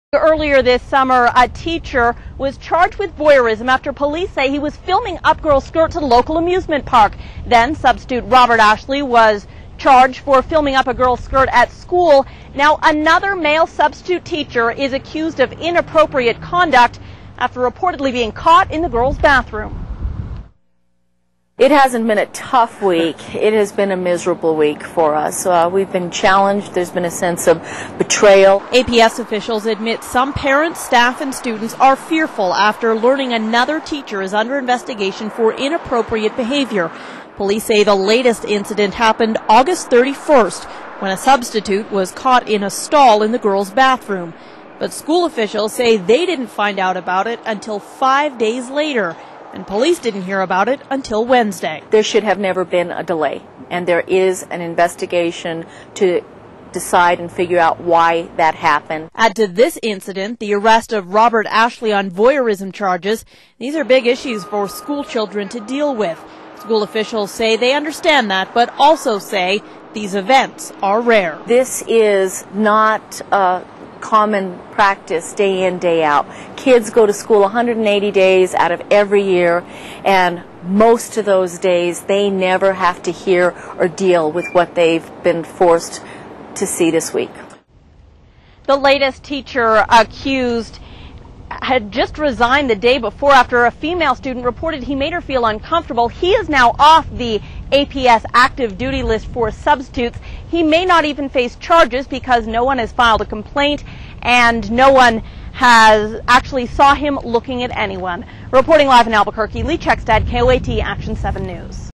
Parents Raise Concerns to APS
APS said they admit some parents, staff, and students are fearful after learning about the most recent incident. But officials said these events are rare.